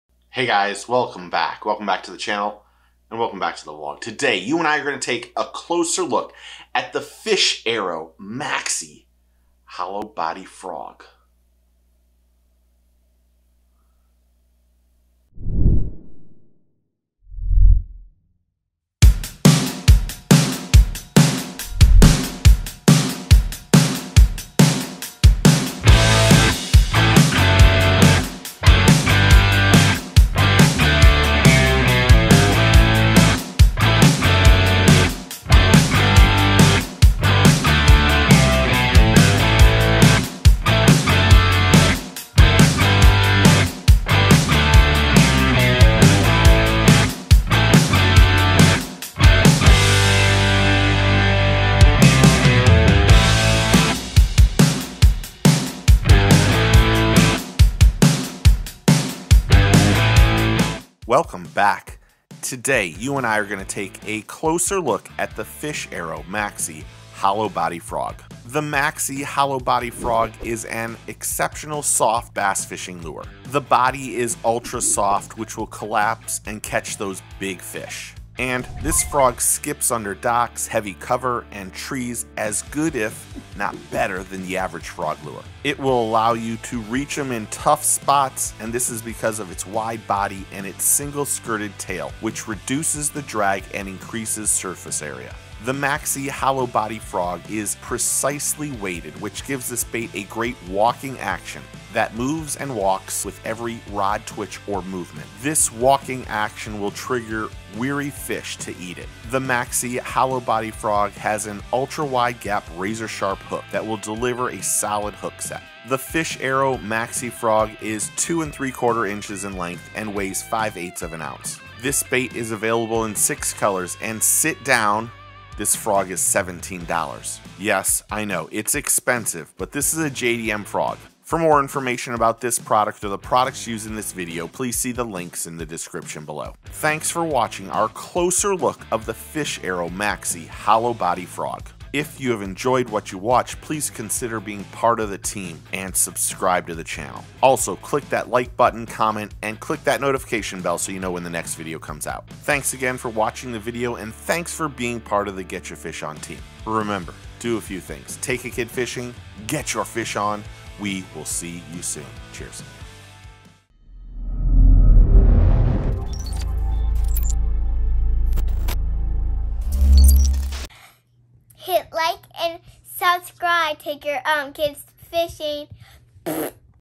The Next GREAT Hollow Body Bass Fishing Frog? Fish Arrow Maxi Frog
Is the Fish Arrow Maxi Hollow body largemouth bass fishing frog the next great topwater frog on the market? Steve of Get Ur Fish On! takes a closer look at this great topwater largemouth bass fishing lure that has great walking action, skips nicely and has good action.  Designed to be snagless, razor sharp hooks and collasable body make this one a great topwater bass fishing frog for anglers.